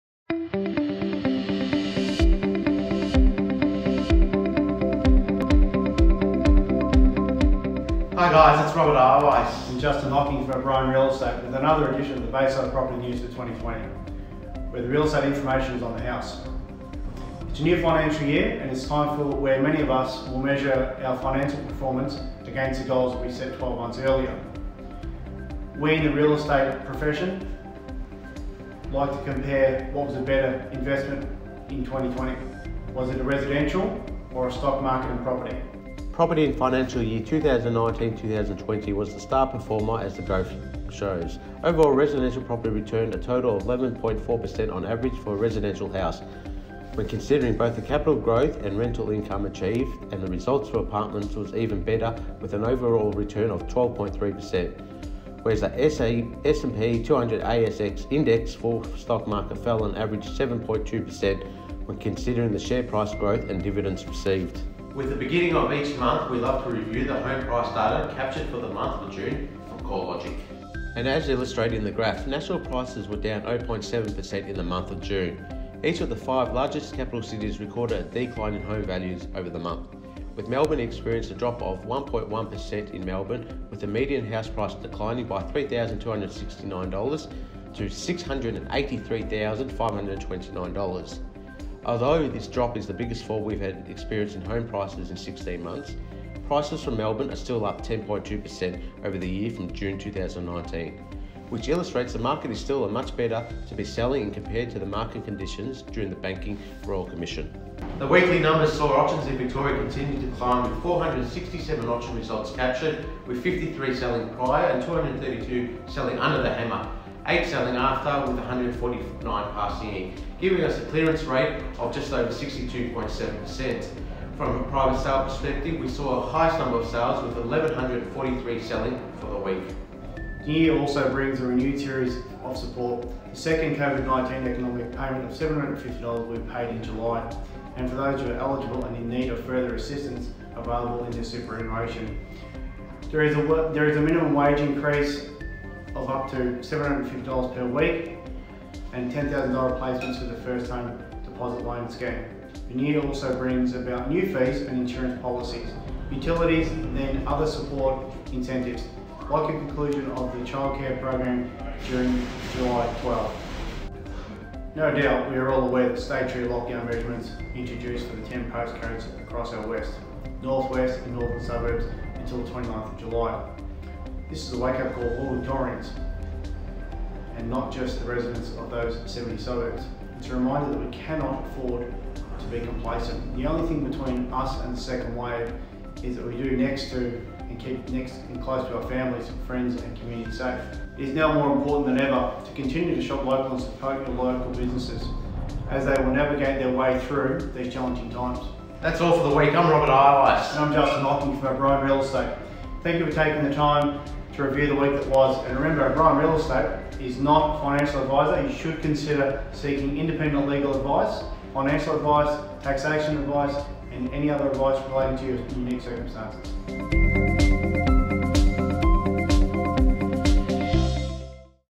Robert Eierweis and Justin Hocking presents Bayside Weekly video of 2nd July 2020
For the latest market and economic insights, auctions and sales in the Bayside area. For more information please call Robert Eierweis on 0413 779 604